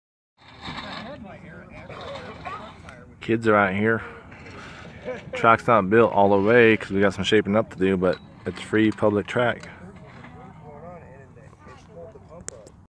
Public track-7
As a Bicycle Picker Yes i help build up the BMX community around here in meany ways.One way with this public BMX track called Youth On Track Cycling in Toltec Arizona.